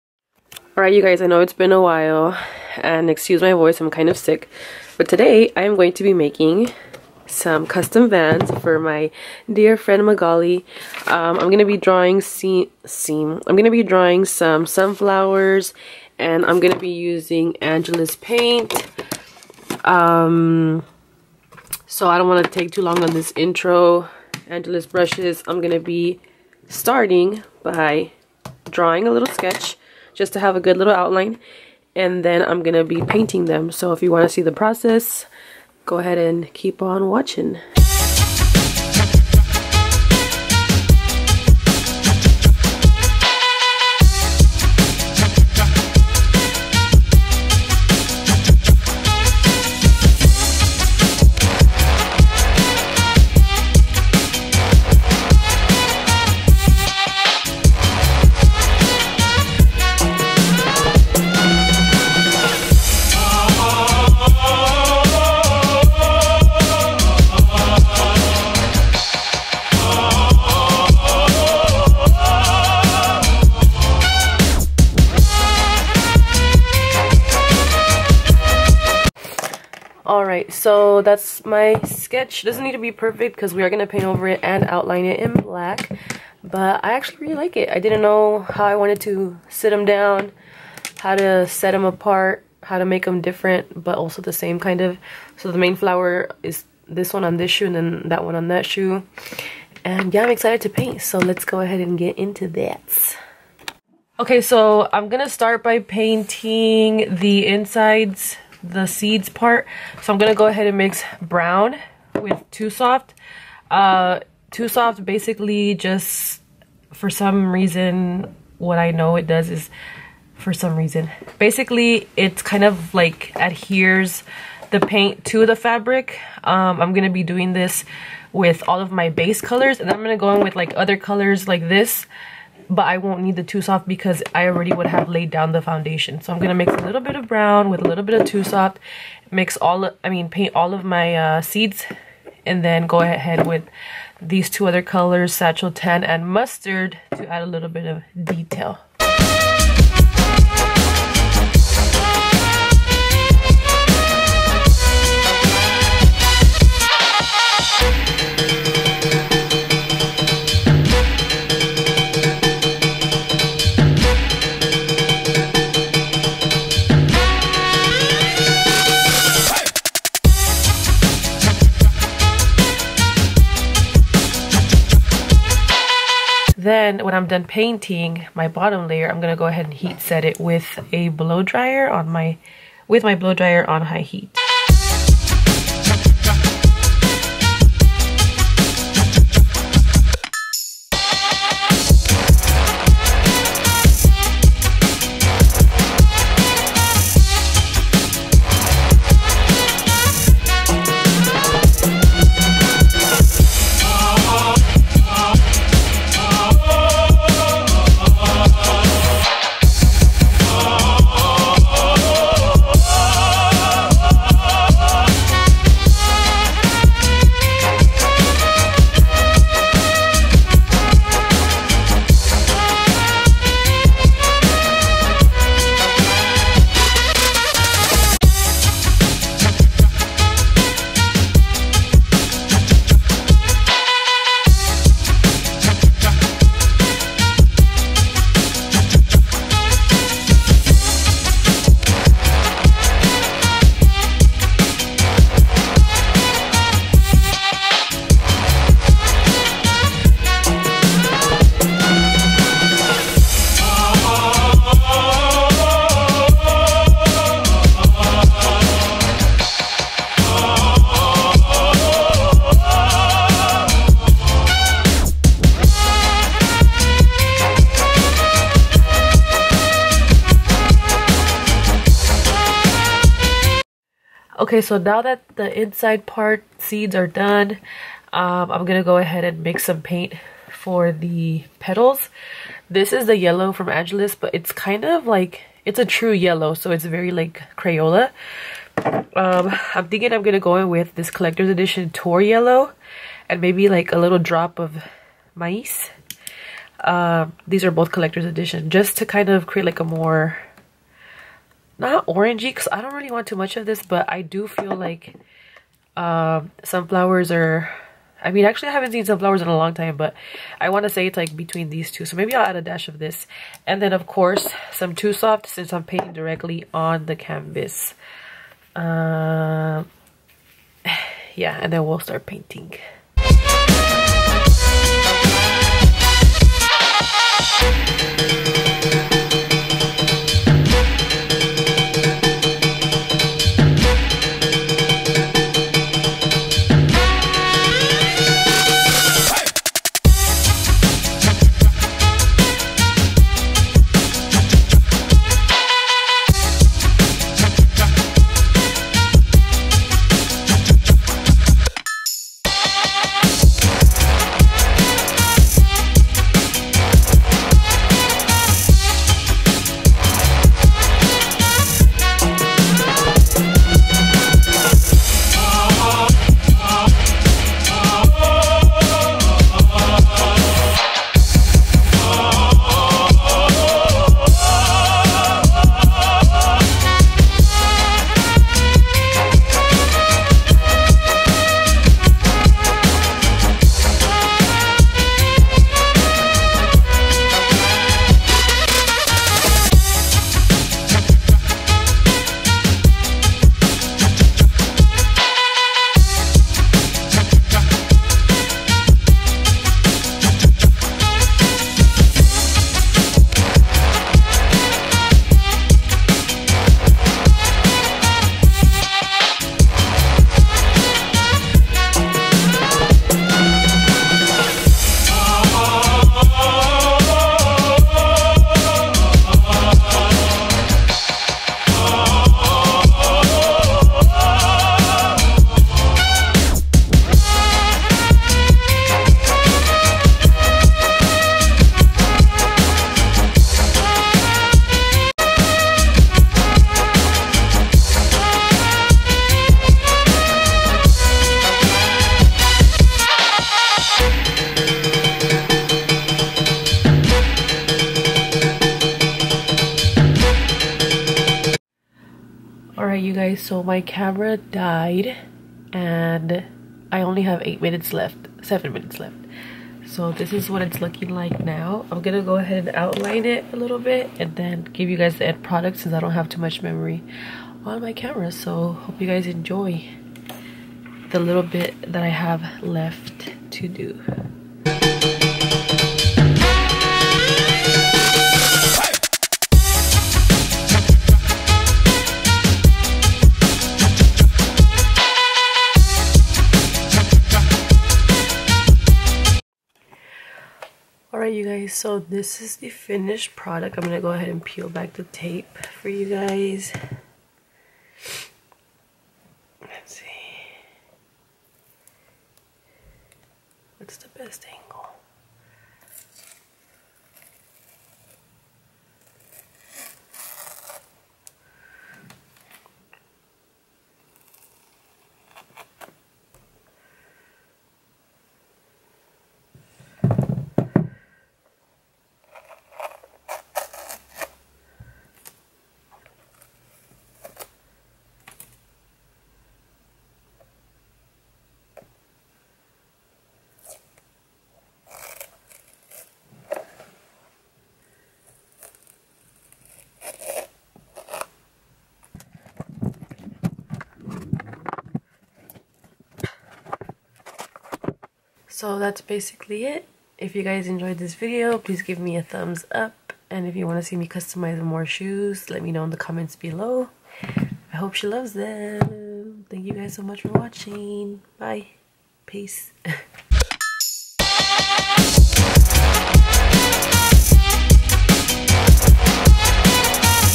How To Paint Sunflower Shoes! • VICEESME
Made some shoesies for my friend Mag so I figured I’d document the process.